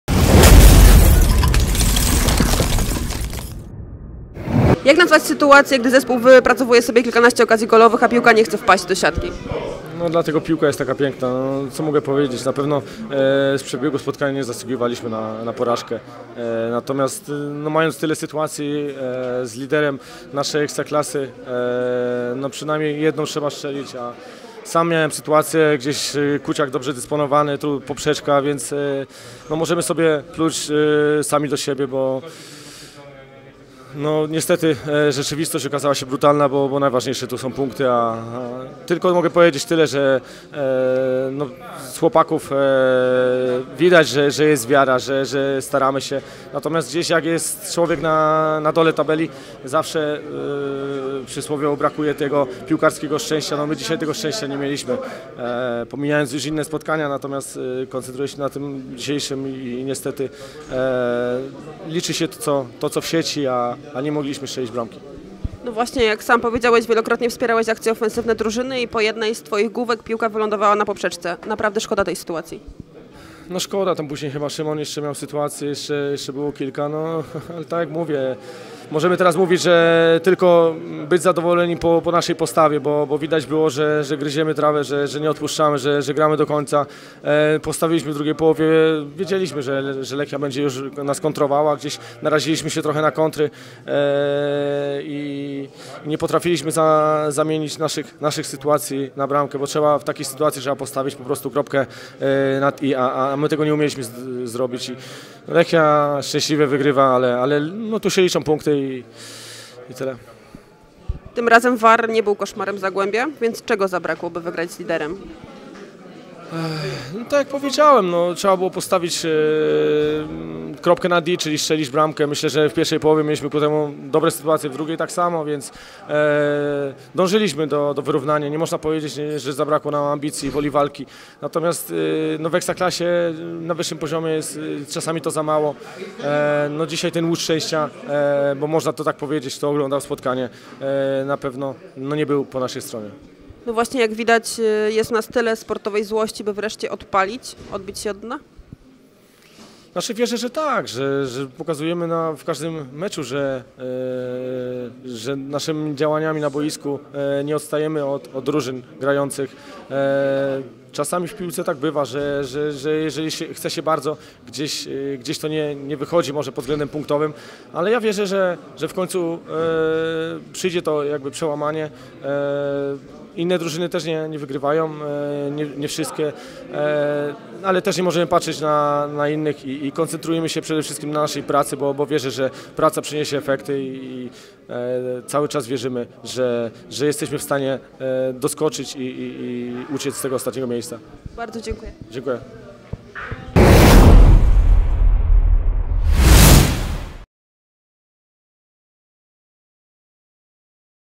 Piotr Polczak po meczu z Lechią
Przedstawiamy wywiad z Piotrem Polczakiem po przegranym 1:0 meczu z Lechią Gdańsk.